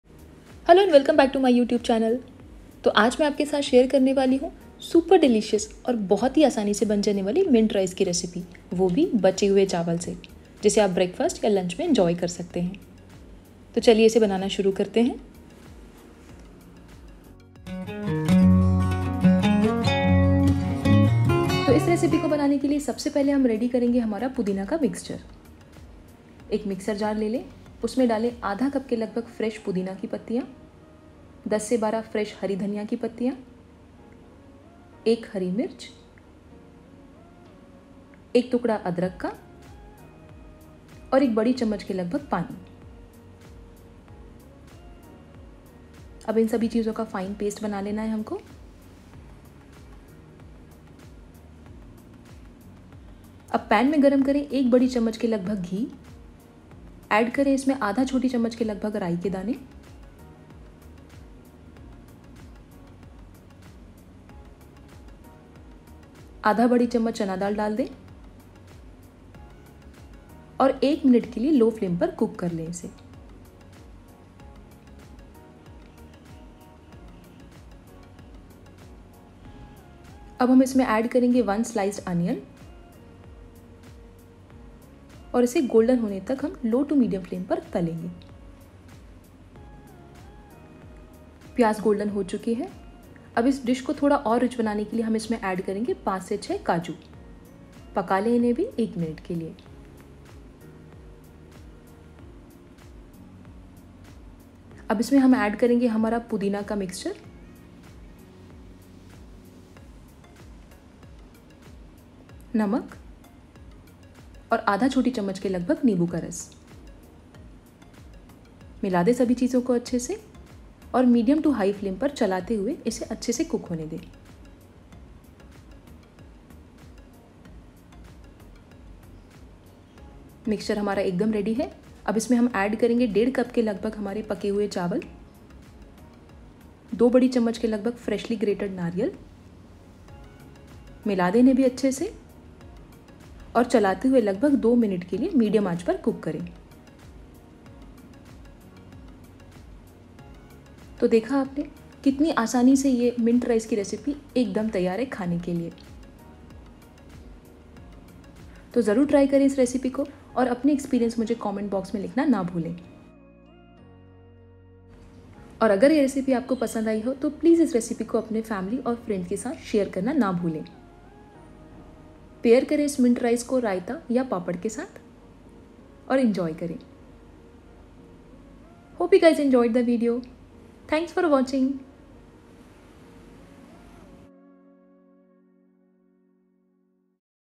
Mint Rice | Pudina Rice recipe | चटपटा पुदीना राइस बनाने की विधि
Aaj ke is video mein hum aapko batayenge bache hue chawal se mint/ pudina ko banaane ki vidhi. ye rice banana bahut he aasan hain aur behad tasty hai.

Ingredients
1/2 cup fresh mint leaves
10-12 fresh coriander leaves
1 green chilli
bud of ginger
1 tbs water
1 tbs ghee
1/2  tsp mustard seeds
1 tsp chana dal
1 onion
5-6 cashew
2 tbs freshly grated coconut
salt
1 and 1/2 cup cooked rice
1/2 tsp lemon juice